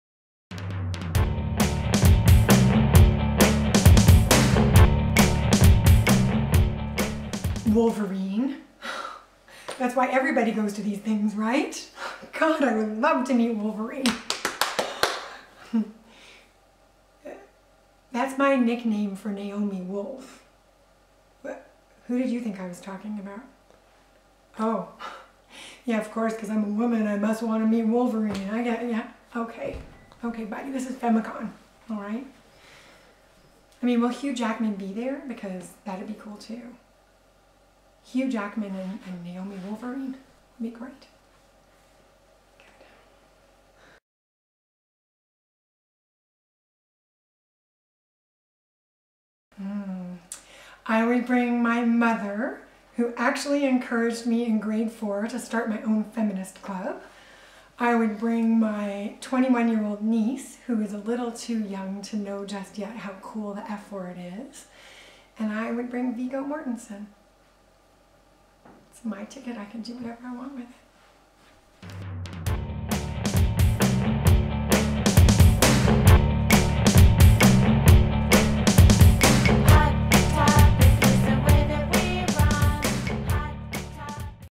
FEMICON trailer (featuring Geri Hall)
Celebrate International Women's Day with "FEMICON"
Free public event on March 8 2013, doors open at 6:00PM
@ Library and Archives Canada auditorium, 395 Wellington

The evening will include an edgy and engaging live talk-show styled panel discussion
on the feminist year in review, a short film featuring many local feminists and Geri Hall and the fifth annual Femmy Awards,which recognize the contributions of local women activists in the National Capital region. Music by DJ Jas Nasty, light refreshments and a cash bar will be available before and after the event.

Ottawa-based organizations have joined forces to presents the fifth annual International Women's Day event to celebrate over a century of achievements, to make feminism fun and relevant and highlight the connections between local, national and international women's rights issues.

Hosted by:
Amnesty International Canada
Canadian Federation of University Women (CFUW)
Inter Pares
Oxfam Canada
Planned Parenthood Ottawa
World University Service of Canada (WUSC)
And more!